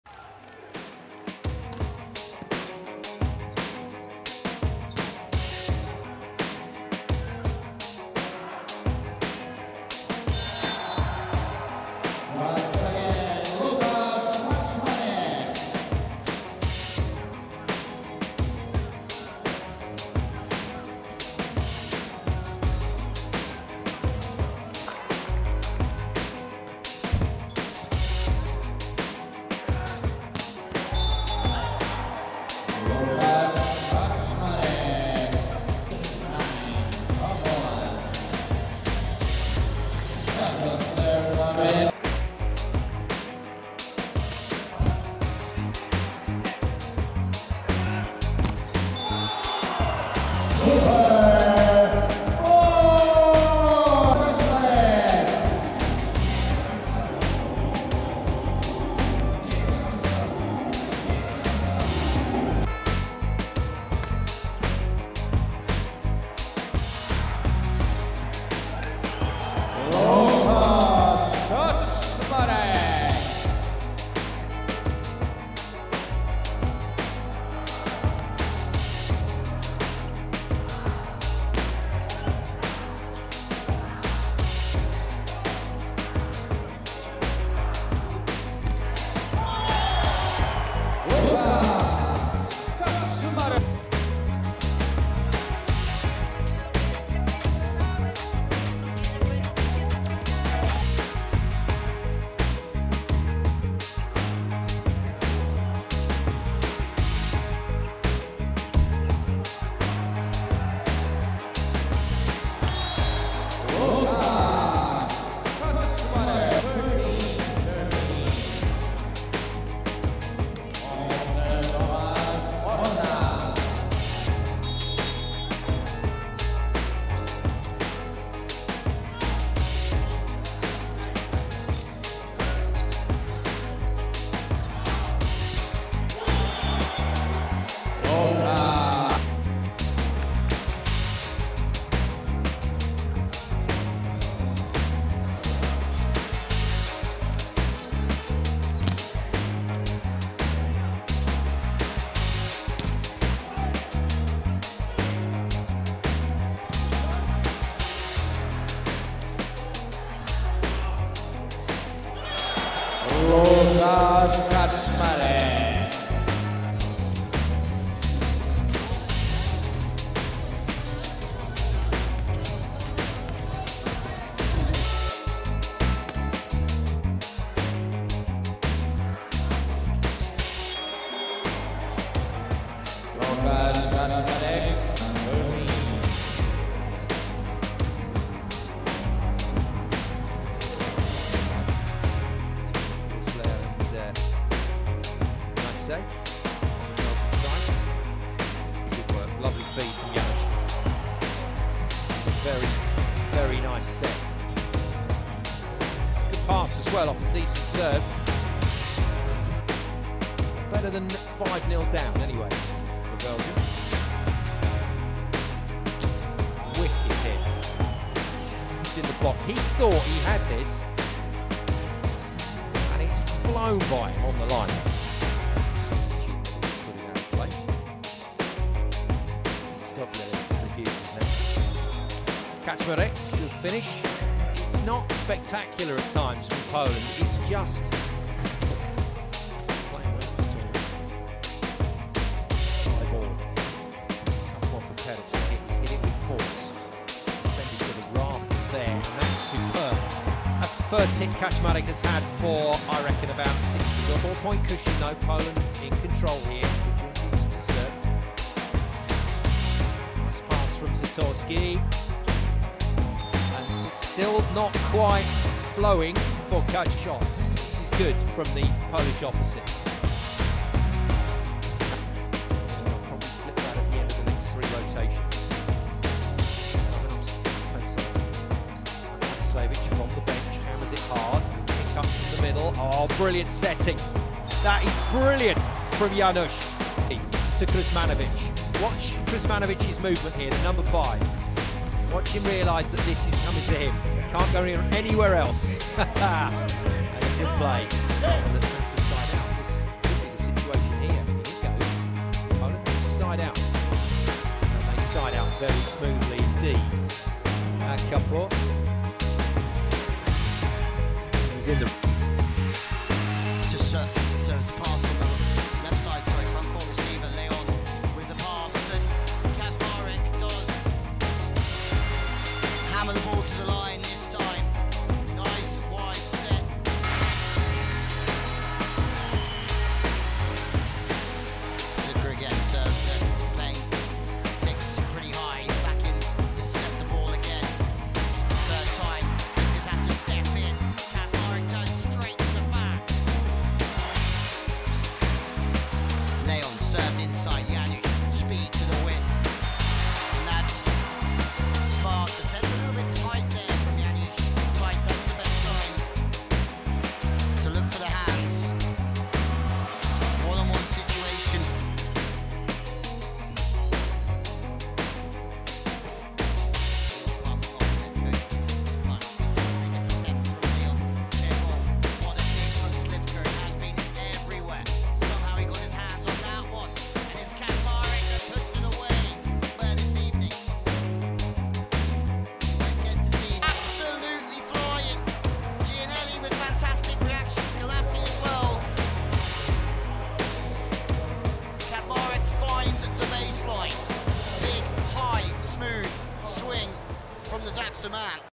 Łukasz Kaczmarek's Volleyball Artistry: A Diverse Arsenal of Attacks!
🏐 Experience the exceptional talent Łukasz Kaczmarek of one of the world's best volleyball players, Łukasz Kaczmarek, in this captivating compilation!

Join us as we highlight Łukasz incredible skills in blocking, attacking, and serving. Each clip in this video is a testament to his remarkable abilities on the court.